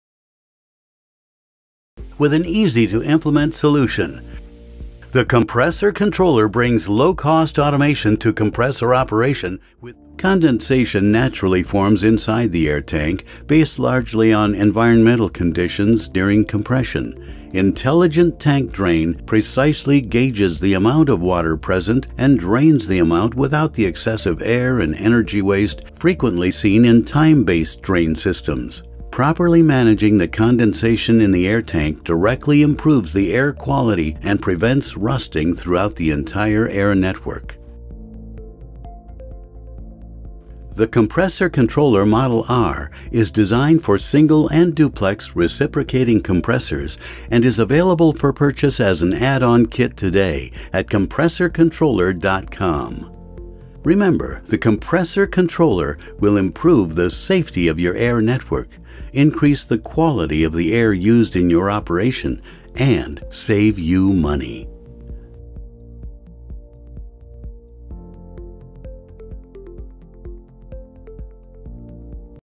Air Compressor Auto Drain
Automatic draining of water from air compressor